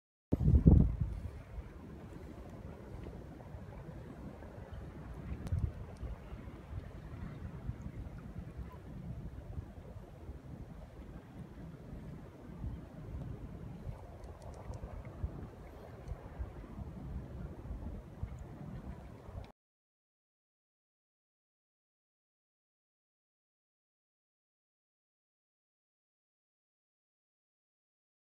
Halibut from Avila Beach
Catching halibut from touring kayak; a bit tight, but fun.